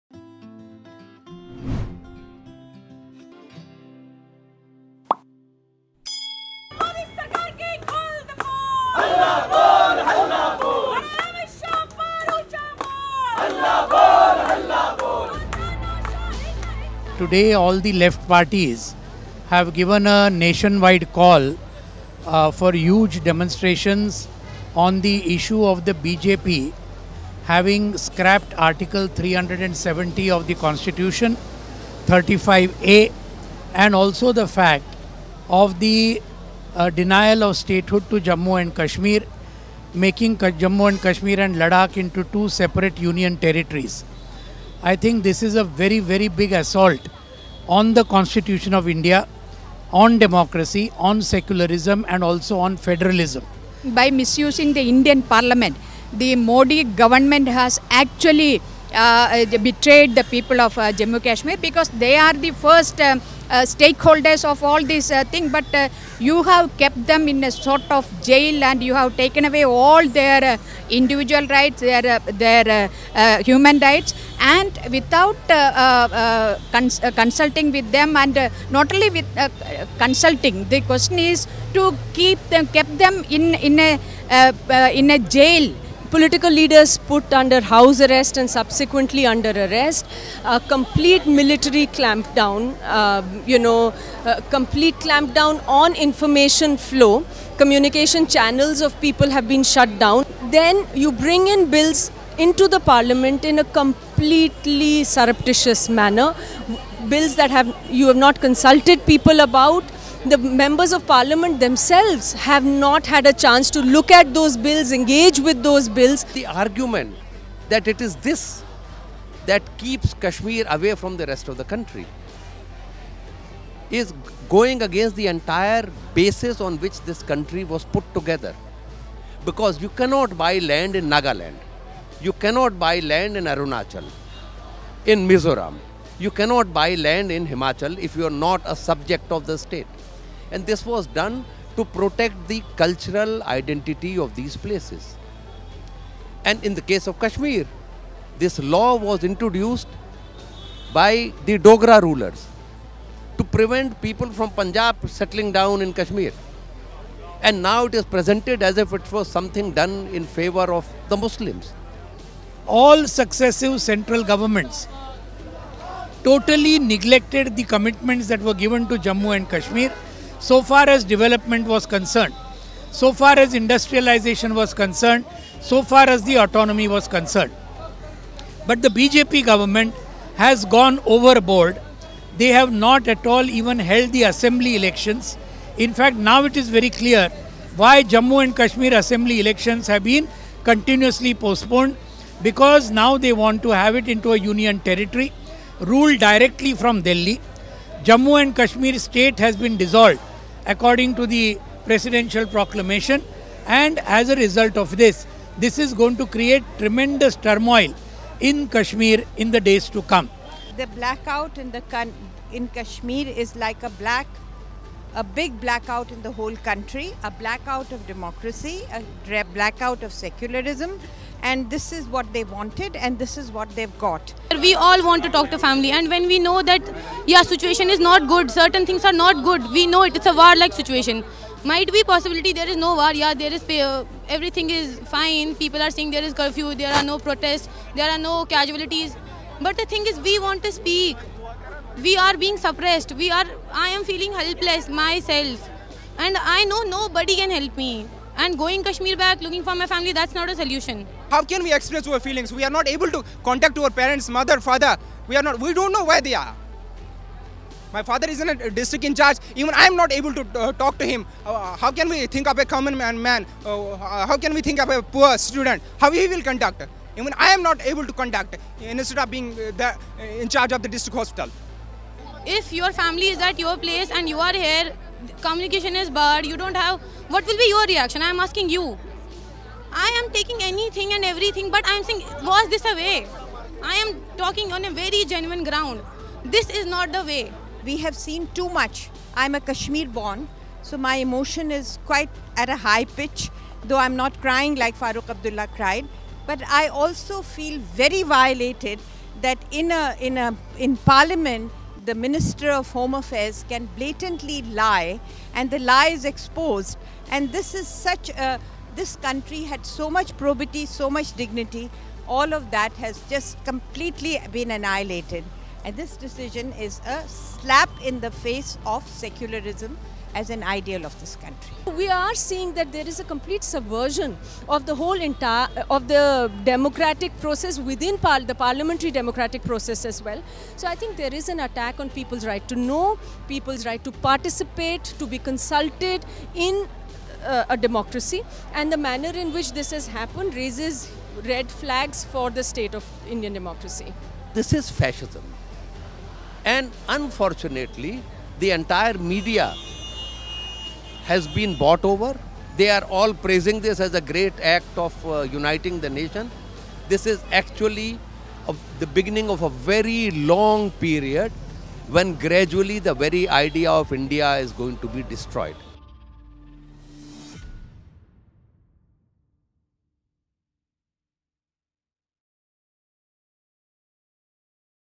Left Parties Protest in Delhi Against Abrogation of Article 370
In a show of resistance following the revocation of Article 370, Left parties continued their protest for the third day on Wednesday.